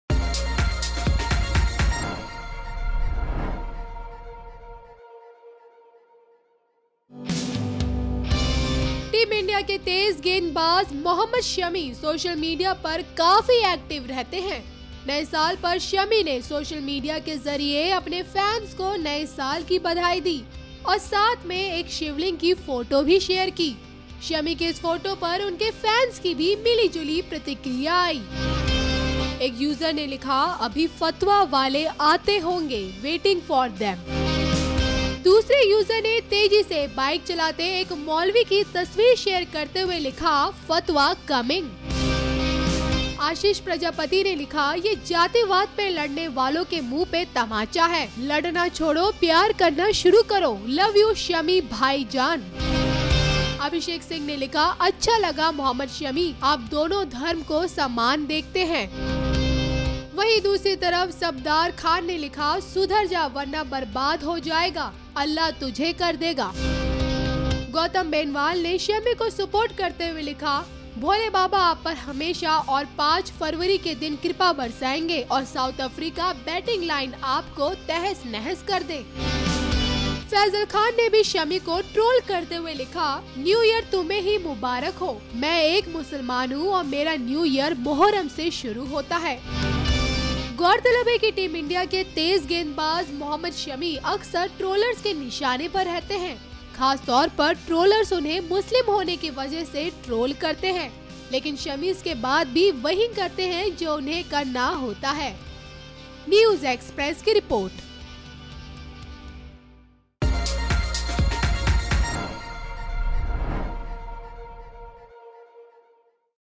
Cricketer Mohammed Shami ने Share की Shivling की Photo, Fans ने दिए ऐसे Reaction
Cricketer Mohammed Shami ने New Year पर Shivling की Photo Share की है हालांकि इस Photo के लिए कई लोगों ने उनकी आलोचना भी की तो कई ने उनकी तारीफ की.